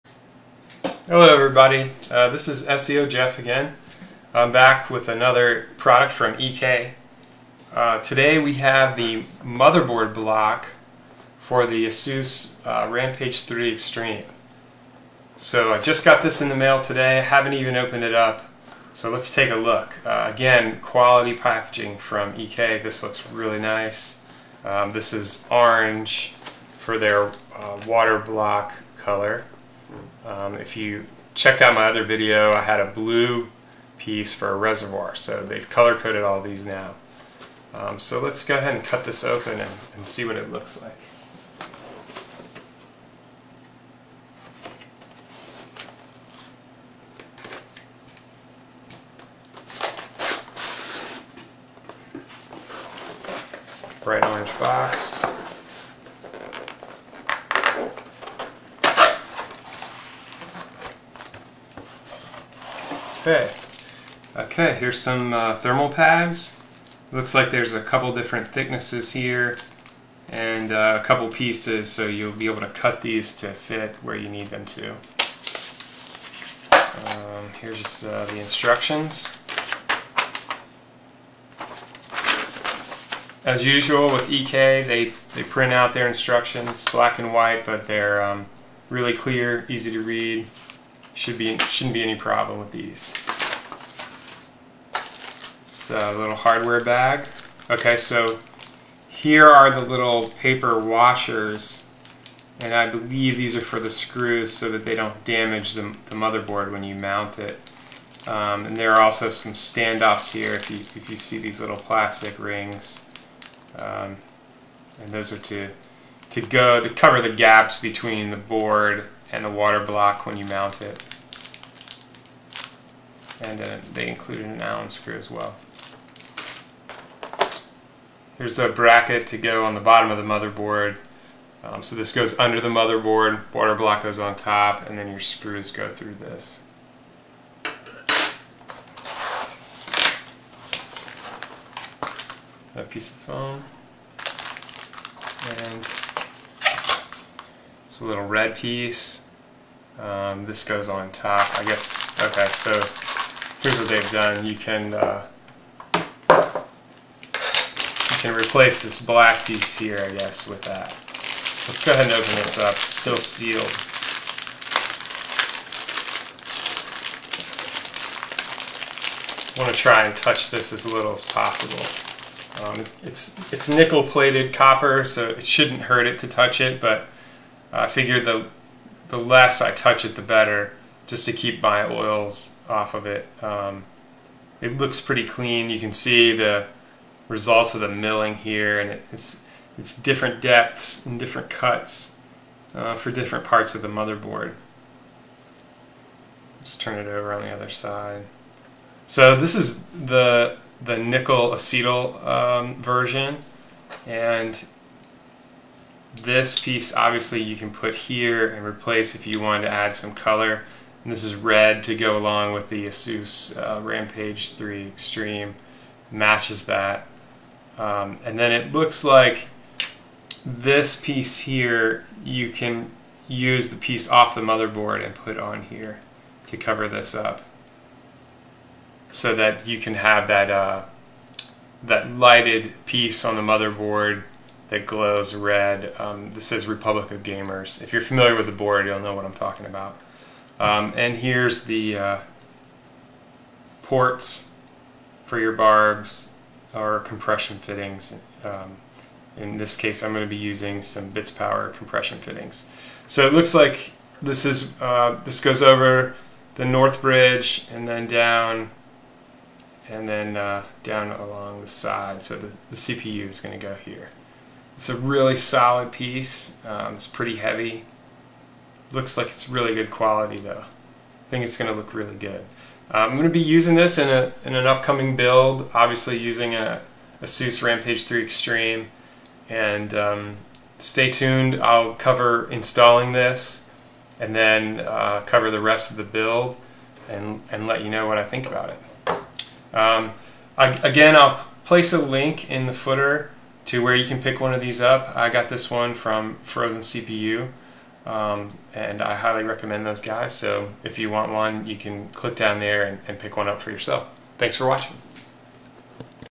EK-F8 RE3 ASUS Rampage III Extreme chipset waterblock unboxing
Unboxing the EK motherboard chipset water cooling block for the ASUS Rampage III Extreme.

EK-FB RE3 Acetal+Nickel Acetal water block is a complete solution for ASUS Rampage III Extreme motherboard. Block effectively cools NB, SB and mosfet.

Two bases are made of nickel plated electrolytic copper. Tops are made of quality POM Acetal material (EKWB added also redish transparent top level 2 that gives your hardware outlook extra coolness) . The sealing is performed by quality rubber washers. Blocks are mounted with enclosed screws.

Furthermore EKWB design allows user to reinstall ROG connector as it gives improved look of unity and high-tech cooling solution for overclockers.

Enclosed:
-waterblock
-mosfet backplate
-mounting mechanism
-thermal pads
-RED acrylic top lev. 2